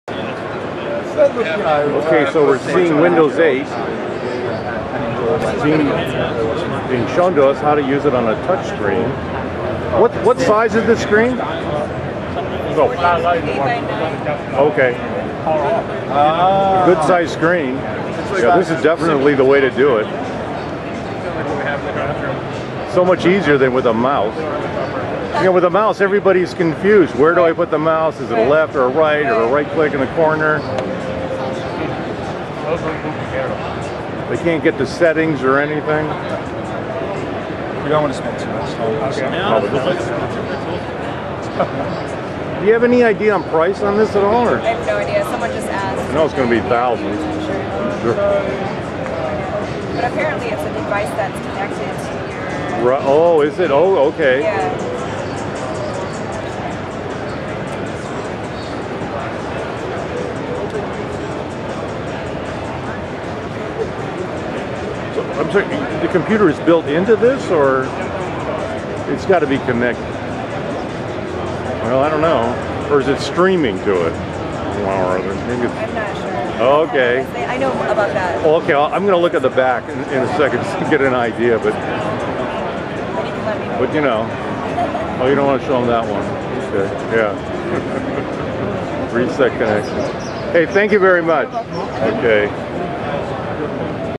TCL Touchscreen with Windows 8
TCL Spokes Model demonstrates Windows 8 using a touchscreen from The Creative Life.